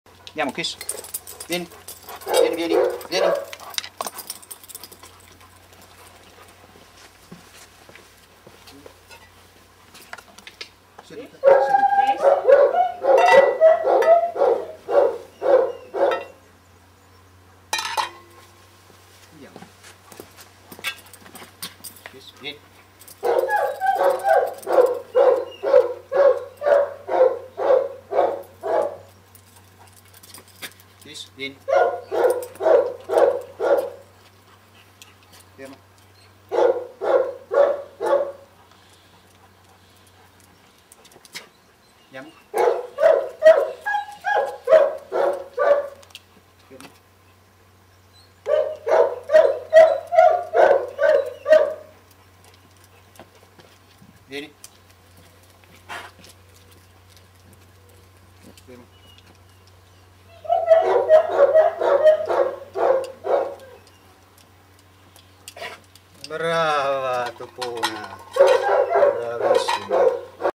Esercizio della "Start line" - IdealDogRE
Condotta al guinzaglio senza tirare per raggiungere un target (cibo/gioco) al passo del conduttore. Gratificare verbalmente il cane per l'esercizio corretto
Esercizio della Start Line
Starring: Rapsody (Kiss)
IdealDog RE Educazione Cinofila